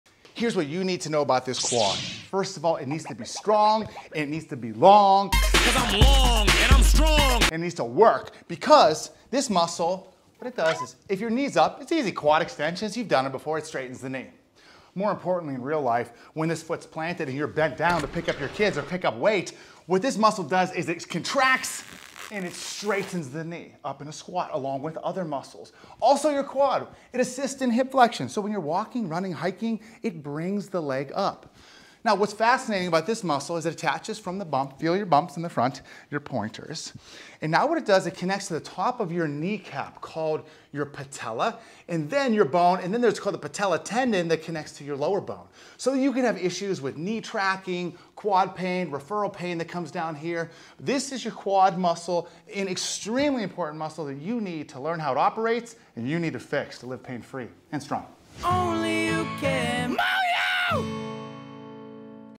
Quad Anatomy
Your quadriceps need to be strong, and they need to work.

The quads are a group of four muscles on the front of your thigh. They attach from the top of your knee to your hip area, crossing both the knee and hip joints.

When the knee is bent and you straighten it, that’s your quads contracting. They’re responsible for extending the knee and helping to stabilize it when you stand, walk, or land from a step.

They also assist in hip flexion—helping lift your leg forward or keep it steady when your foot leaves the ground.

Strong, balanced quads help distribute force evenly through the knee and hip, keeping your joints aligned and your movements controlled.

If your knees cave in, your quads feel tight, or your lower body feels off, start our Valgus Knee & Flat Foot Program to rebuild alignment and get your legs working the way they’re meant to.